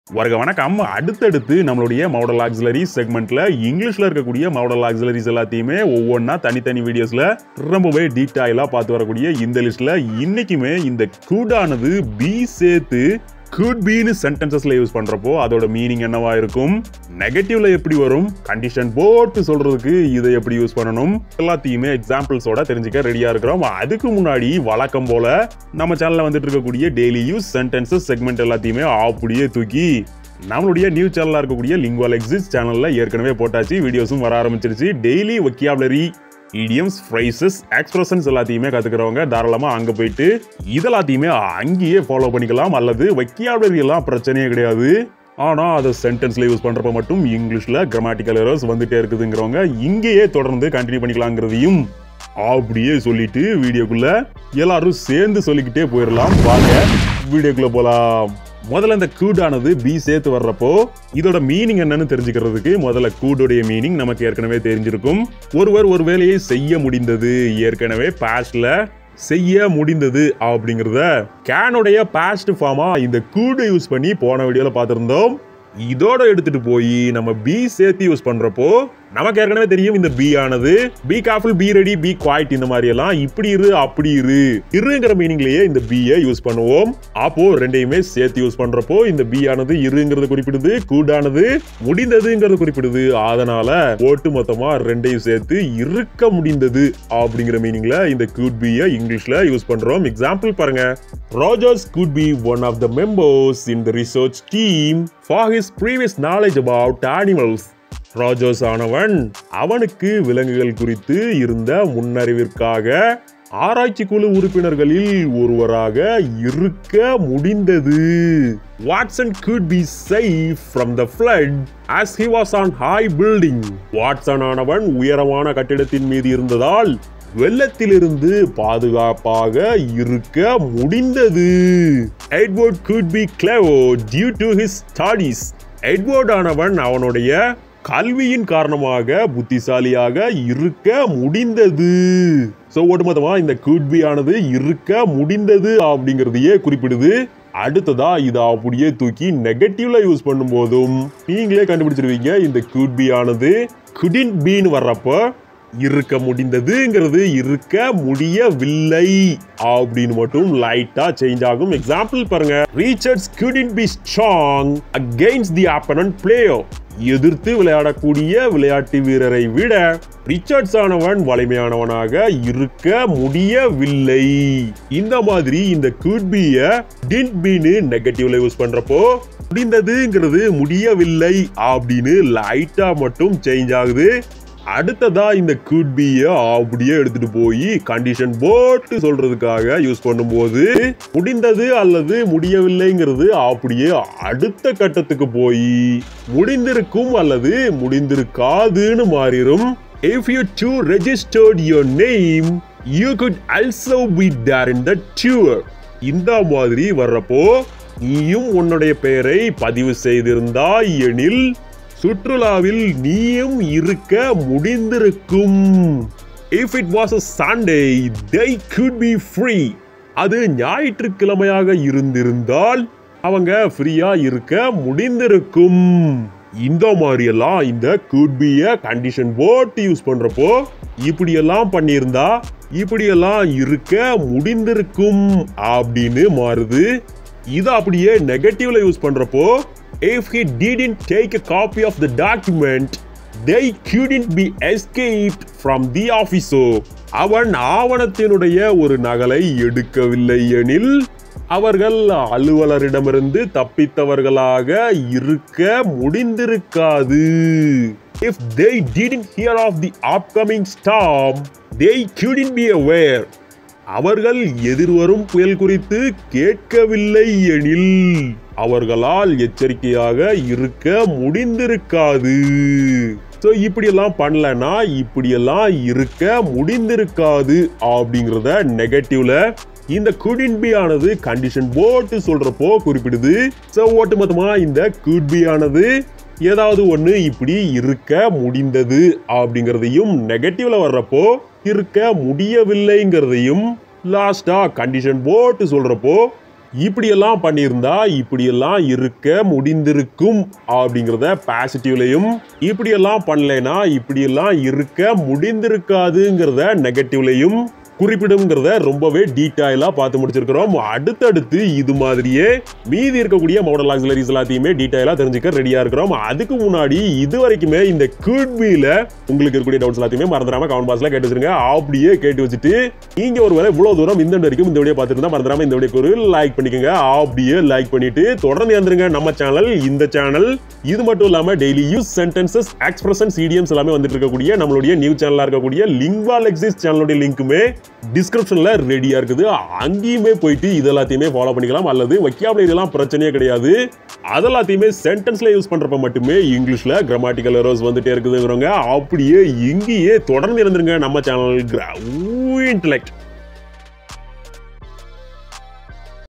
Could be / Spoken English in Tamil, Modal Auxiliaries
Modal Auxiliary Verbs in Tamil (Spoken English in Tamil)
         Hi there! In this series of videos, we are discussing about how to use the Modal Auxiliary Verbs listed below.

Modal Auxiliaries: (Modals) List
     (Will Would Shall Should Can Could May Might  Must)

We will discuss in the upcoming videos of this series about how to use -

Will be
Would be
Shall be
Should be
Can be
Could be
May be
Might be
Must be

There, you can learn the Sentence Formations using the Modal Verbs. Modals are easy to understand, if we exactly study about the mode or meaning that each Modal Auxiliary Verb has.

Pronunciation Practice...

sentence formation